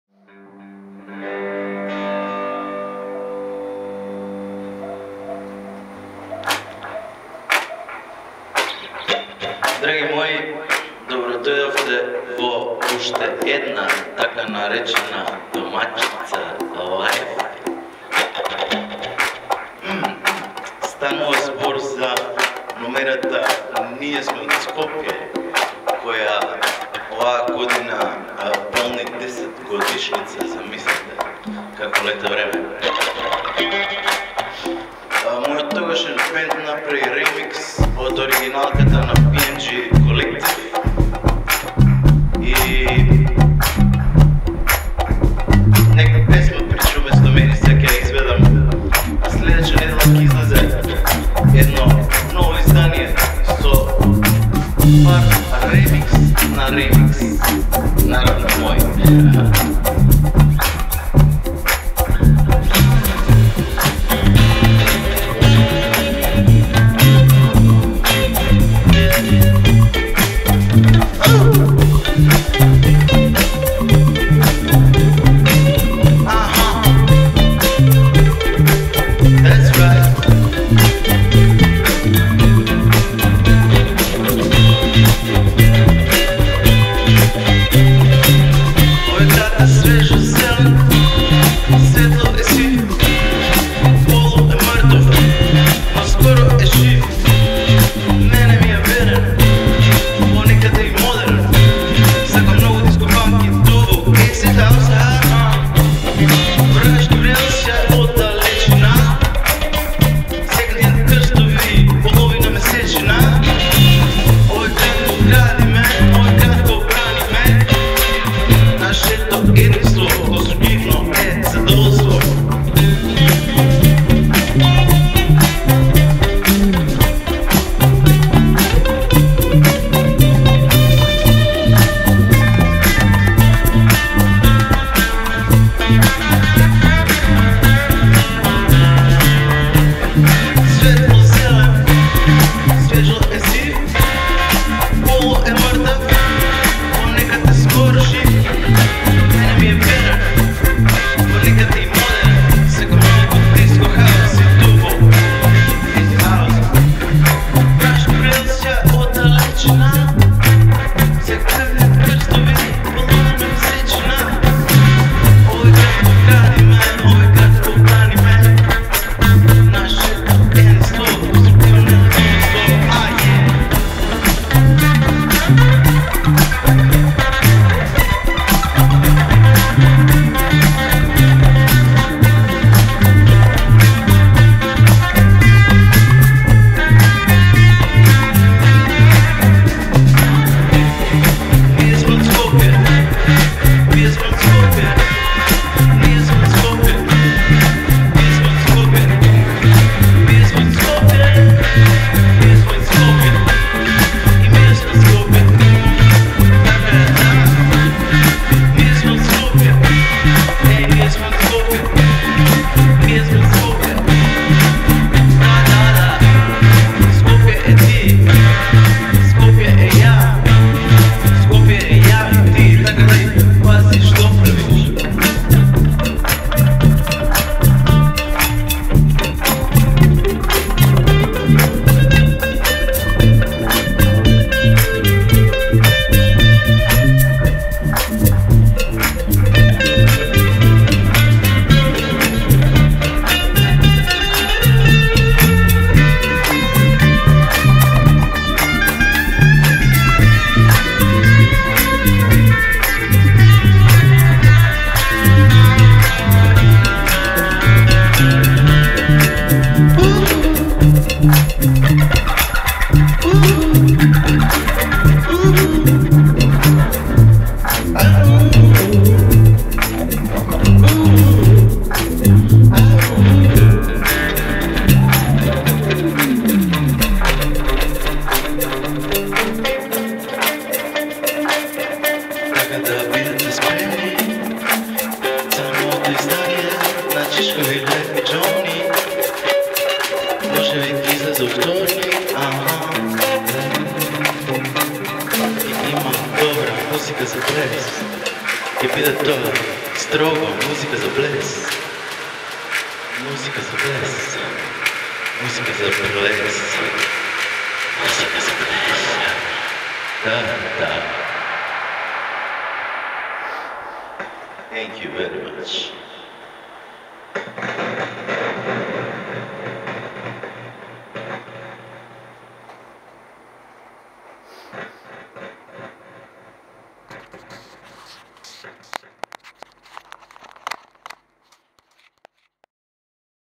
Big City Skopye Xhem @ zu Hause [wegen PolizeiStunde]
as an Audio/Video teaser, 4 the upcoming digital release (for pmg recordings, naturally) , which for me honestly, answers all my dance floor experiences, from the very Dawn of this Milenia, this half an hour audio package, sums it all, atleast 4 me it does, you remember ?? When them dj's swap them turntables 4 guitars an the other way around. Them times, me be bring 'em on Home, them times to U